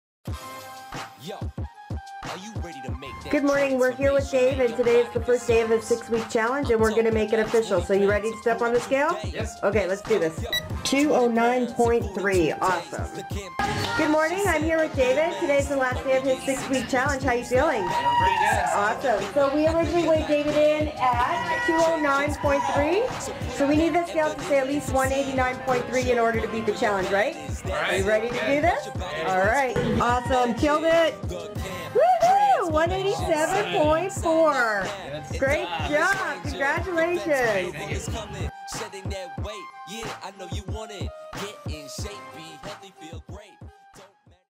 Costa Mesa Fitness 6 Week Challenge Result - David Scribner
We will now check the result of David's 6 week weight loss challenge!